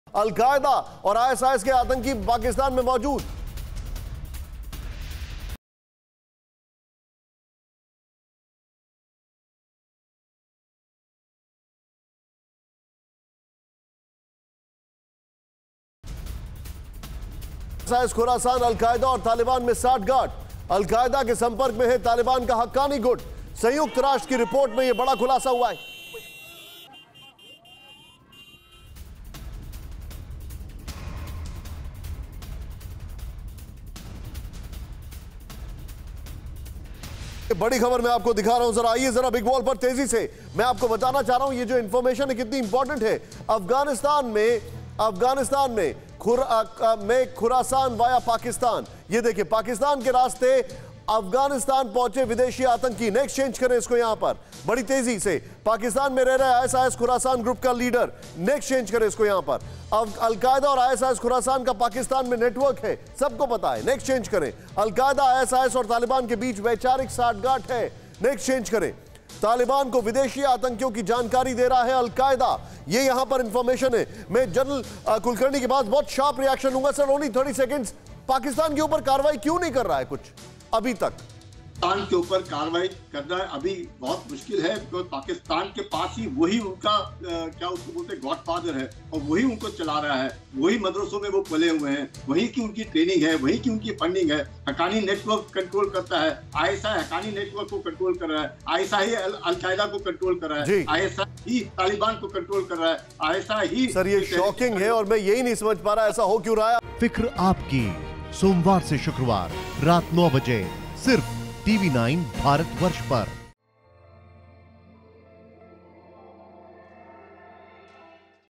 Pakistan के रास्ते Afghanistan में घुसा ISIS Khorasan
पाकिस्तान के रास्ते अफगानिस्तान में ISIS खुरासान घुसा. अलकायदा और IS के आतंकी पाकिस्तान में मौजूद हैं. ISIS खुरासान, अलकायदा और तालिबान में सांठगांठ है. अलकायदा के संपर्क में तालिबान का हक्कानी गुट है. संयुक्त राष्ट्र की रिपोर्ट में बहुत बड़ा खुलासा हुआ है.